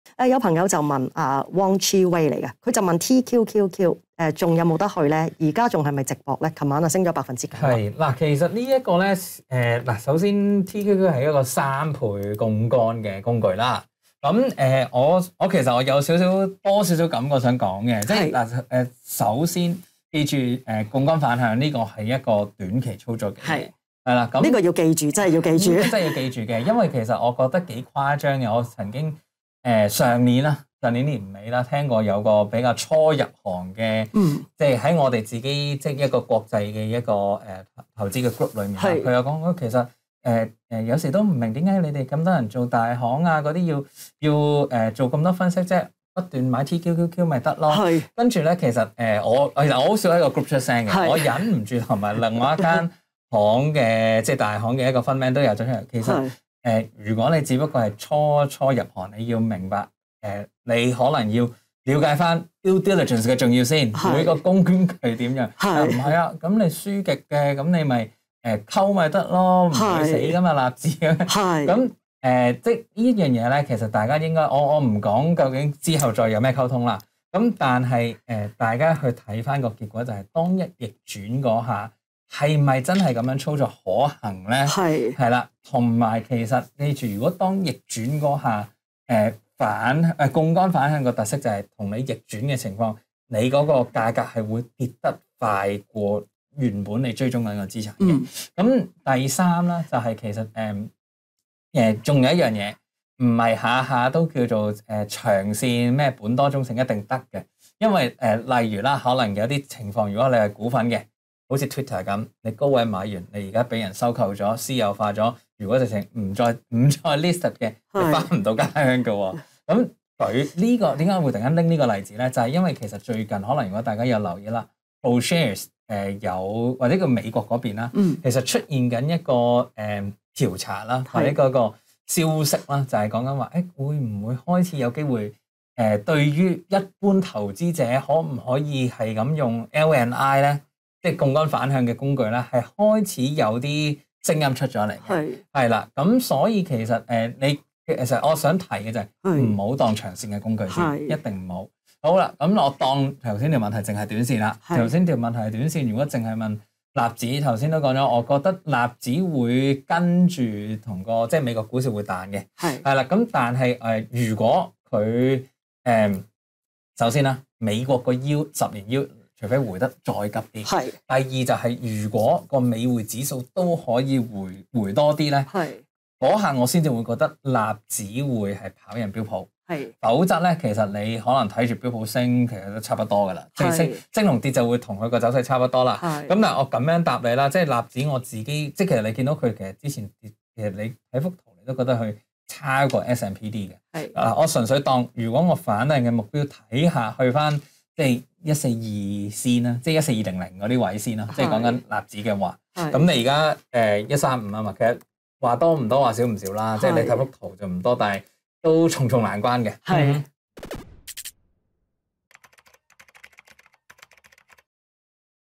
【ET開市直擊】（精華）買TQQQ博美股升？3大注意事項+納指目標話你知
美國聯儲局加息半厘符合預期，道指飆升逾900點，標指、納指升逾3%，美國監管機構擬提高複雜ETF 散戶投資門檻，ProShares三倍做多納斯達克指數ETF（TQQQ.US）可買入博美股升？利達LW基金經理/合夥人黃耀宗（Andy）分析指，買TQQQ有三大注意事項：

（1） 切記槓桿工具只可短期操作，不宜當長線工具

（2） 一旦逆轉價格跌得快於原本的資產，不能靠溝貨

（3） 長線不一定本多忠勝

黃耀宗指，美國已開始有聲音討論對一般可否操作槓桿反向工具，因此再籲投資者不宜當長線工具。他料納指會跟美股彈，反向目標初步睇14200點，但難關重重。

*重溫《ET開市直擊》美國議息結果解讀　北水歸位港股見真章？*